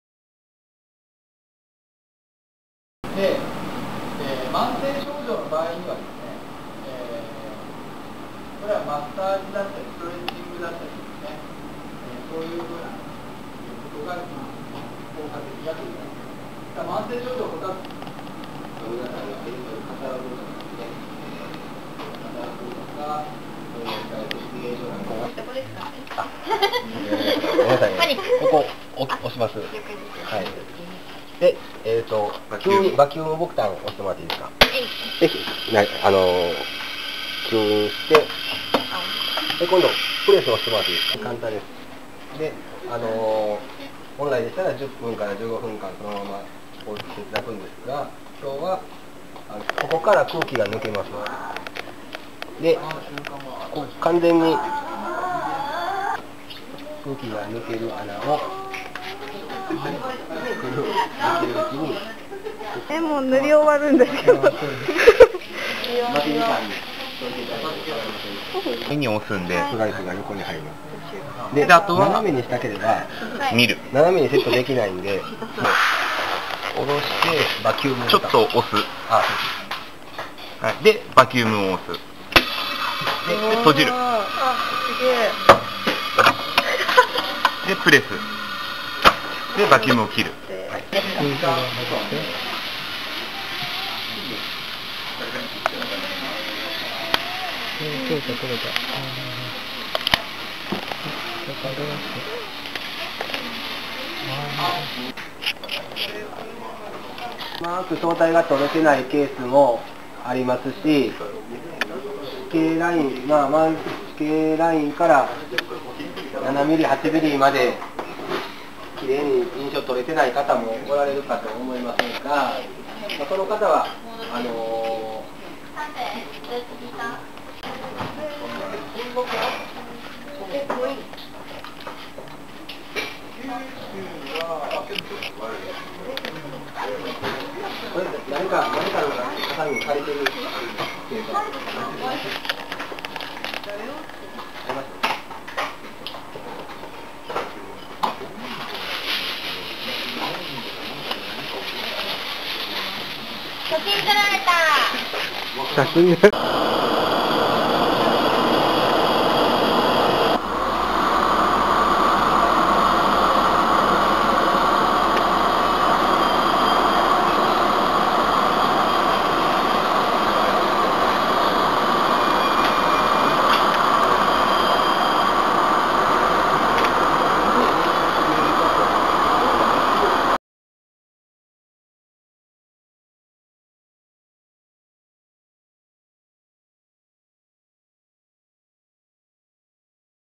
溝部学園 大分県歯科技術専門学校 マウスガードを造る
メーカーセミナー
６月４日(木)
歯科技工科二年生を対象にマウスガードのセミナーが行われました。
午前中はスポーツ歯学の講義があり、午後からマウスガード製作の実習となりました。
マウスガードは、ボクシングやラグビーなど身体が激突する競技において顎口腔系を衝撃から守るために装着する防具です。
学生はメーカーの方の指導に添って興味深く楽しそうに取り組んでいました。